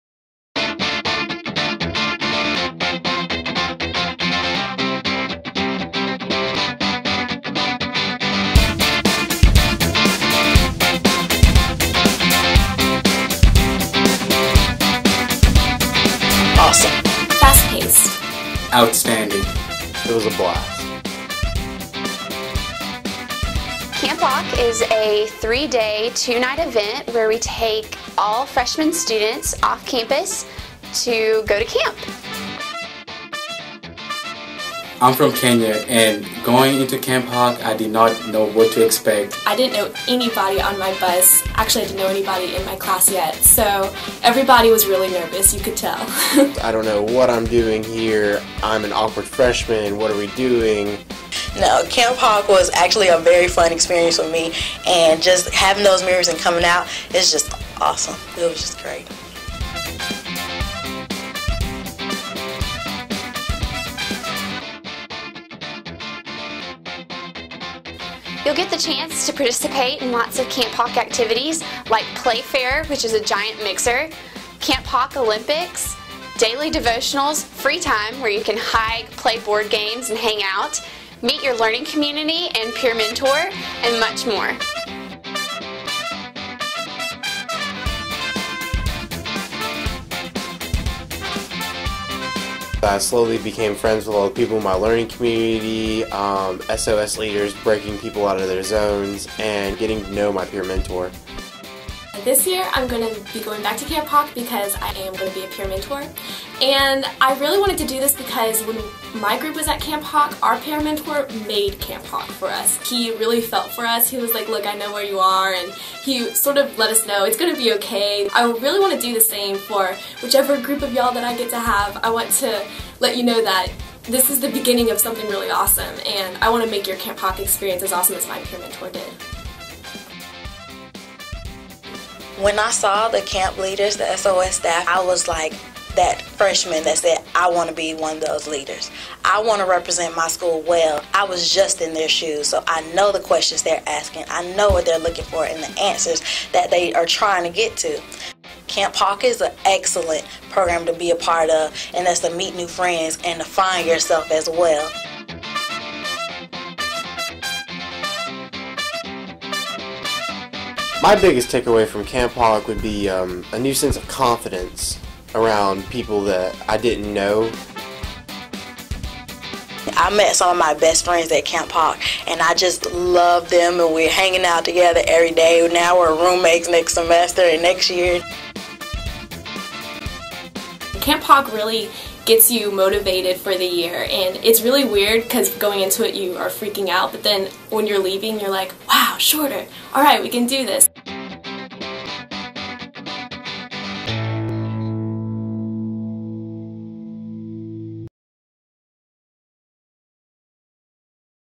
What is Camp Hawk?
This will give you some insight about what to expect from your upcoming Camp Hawk experience. Also, don't forget to register for Hawk Orientation